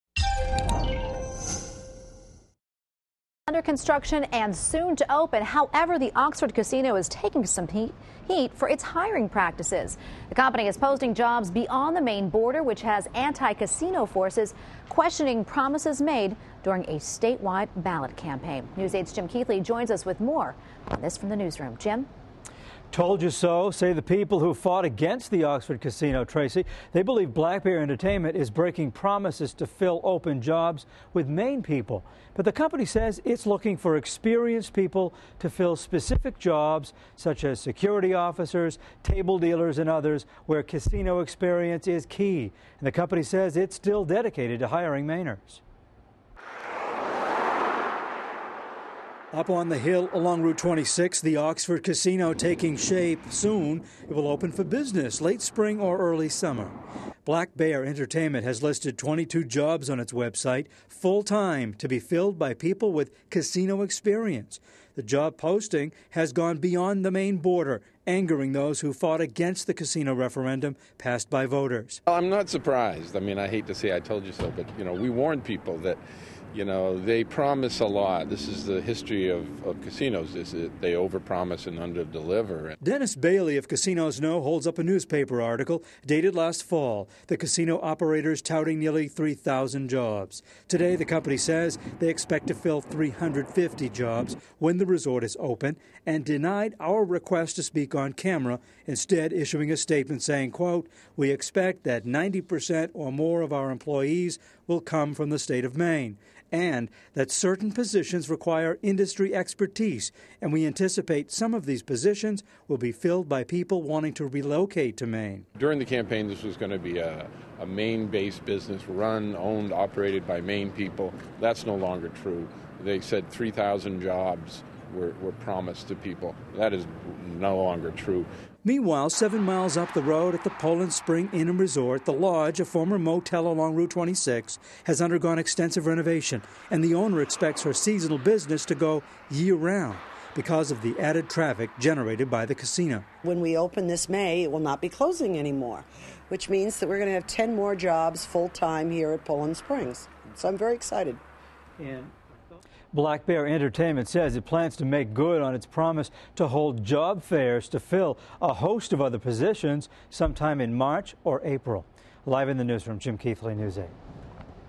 Oxford Casino Begins Hiring Process
The Oxford Casino has created some controversy by looking for employees outside the state of Maine.  News 8's Jim Keithley reports.